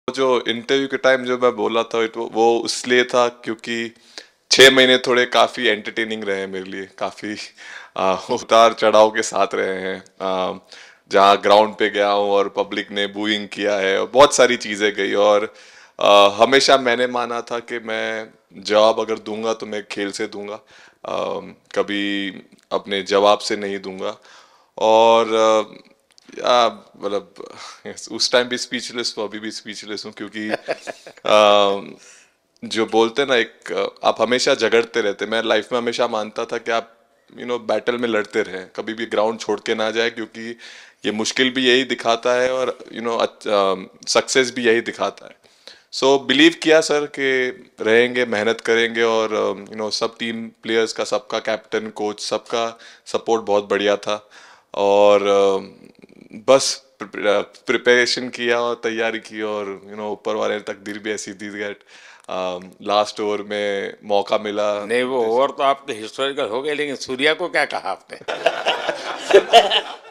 T20 World Cup Champions : Hardik Pandya ने PM Modi को सुनाई अपनी कहानी, बोले- "पब्लिक ने बूइंग किया"
रोहित शर्मा की अगुआई वाली टीम इंडिया ने गुरुवार को नई दिल्ली में प्रधानमंत्री नरेंद्र मोदी से उनके आवास पर मुलाकात की. इस दौरान जब पीएम मोदी ने  हार्दिक पांड्या से उनके प्रदर्शन के बारे में पूछा तो, हार्दिक ने बताया कि किस तरह उनके लिए पिछले छह महीने काफी उतार-चढ़ाव भरे रहे.

About ETV Bharat Delhi:-

You Can follow ETV Bharat Delhi ( ईटीवी भारत दिल्ली ) On:-